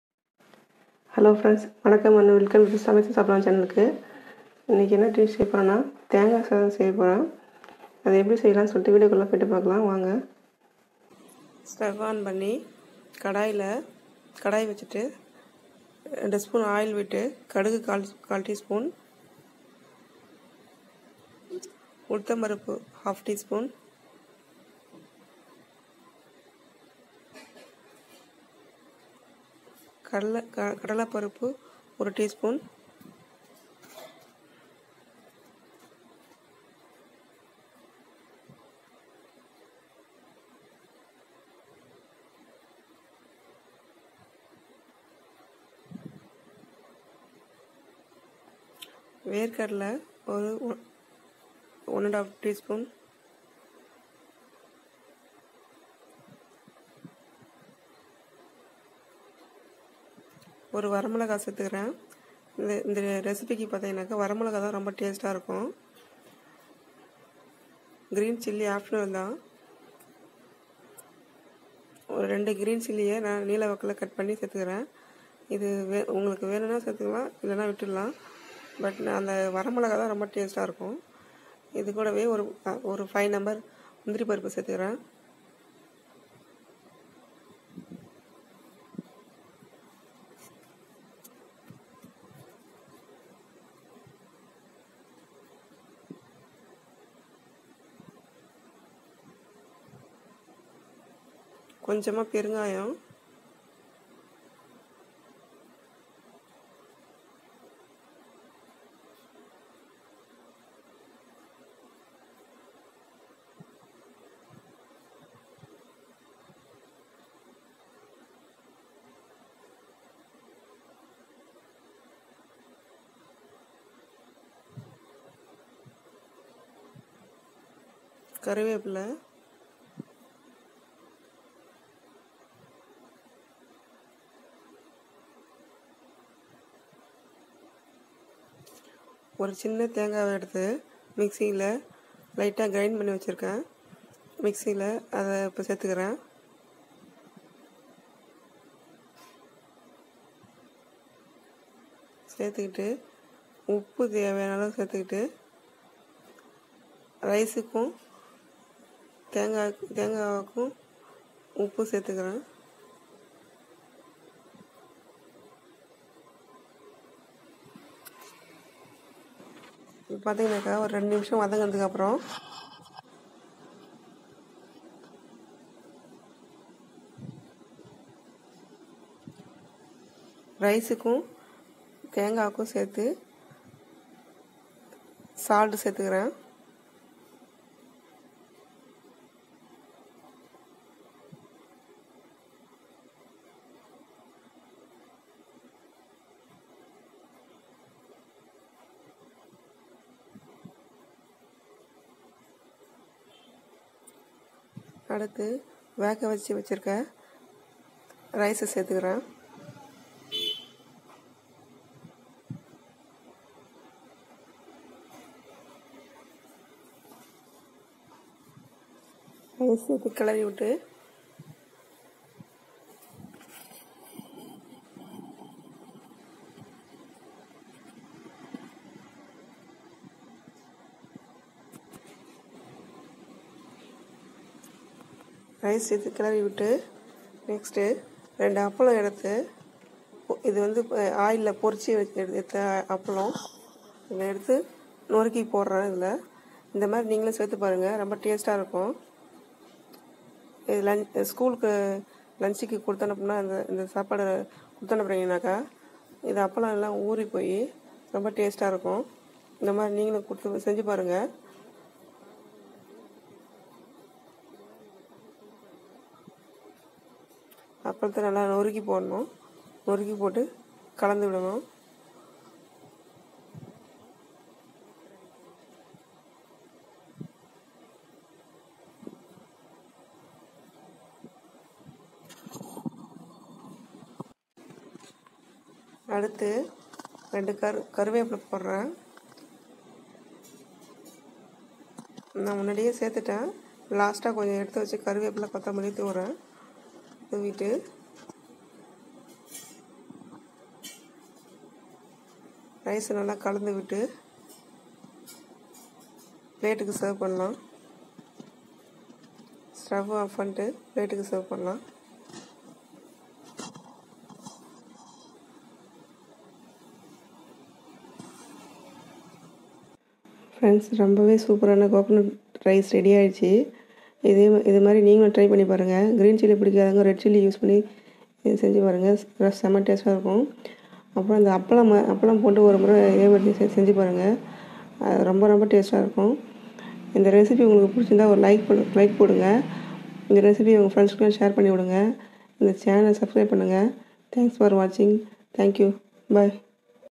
தேங்காய் சாதம்/Coconut rice in tamil/ lunch box recipe
Coconut rice easy preparation

ingredients

rice 1cup 500gm
grated coconut 250gm
coconut  oil 2table spoon

urdu dal 1/2 table spoon
Bengal gram dal 1 table spoon
cashew nuts 5nos
green chillis 2nos
dry red chilli 2nos
Curry leaves 6nos
coriander leaves little bit
asafoetida